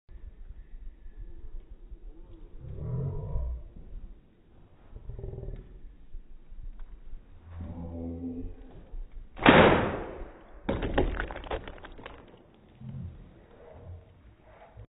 Amazing Bottle Shot (Slow-Motion)
SHOT THE WATER OUT OF THE BOTTLE!